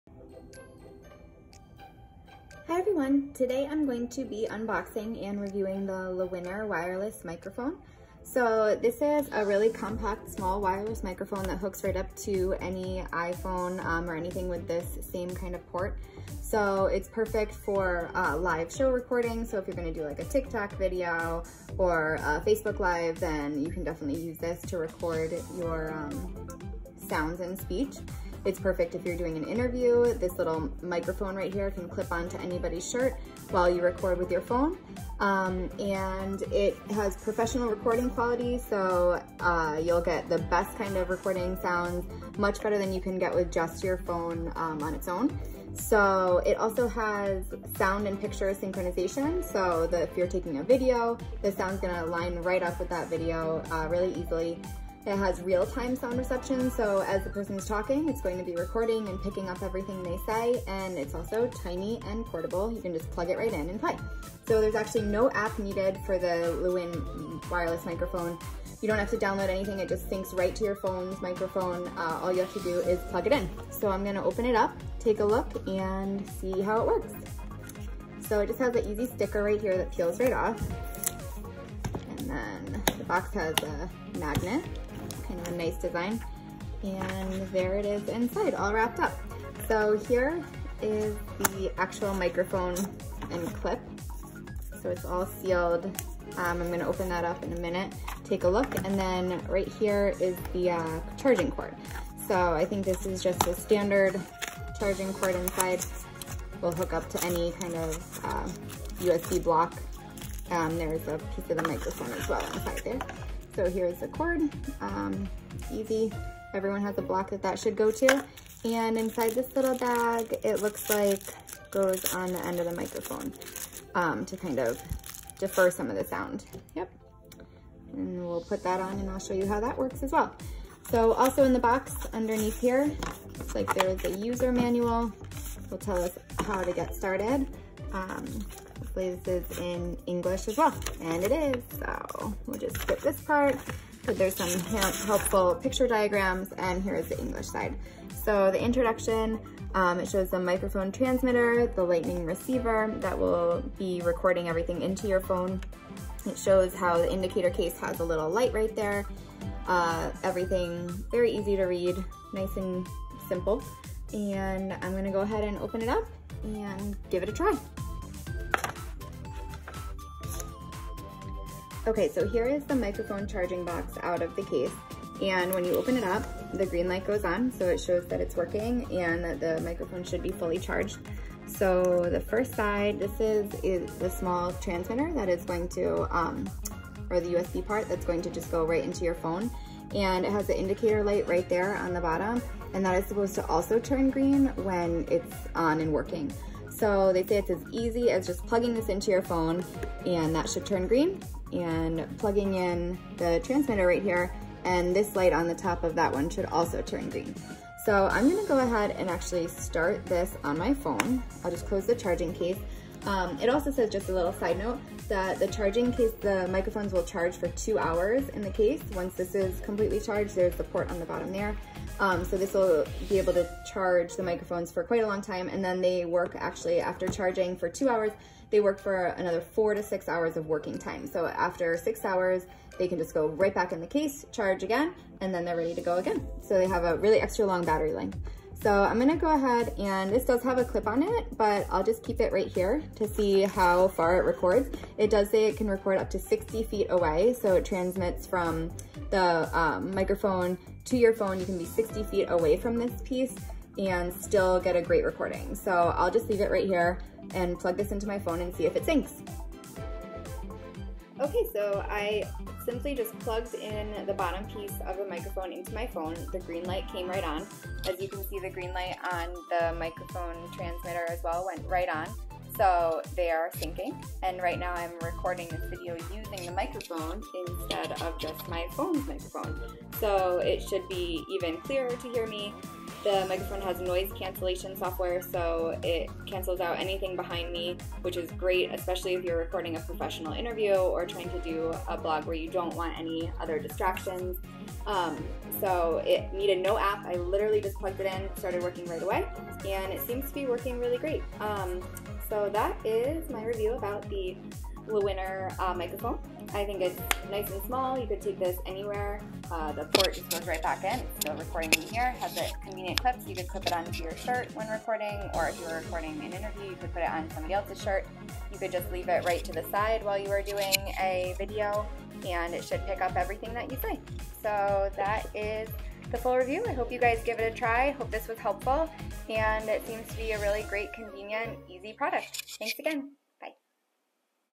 Lewinner Wireless Lavalier Microphone Review | Lapel Mic for Video Recording - Noise Reduction
Lewinner Wireless Lavalier Microphone for iPhone iPad REVIEW: A Long-Term User's Perspective

Lewinner Wireless Lavalier Microphone for iPhone iPad, Plug & Play 2.4GHz Lapel Mic with Charging Case for TikTok YouTube Facebook Live Stream Vlog Video Recording - Noise Reduction/Auto Sync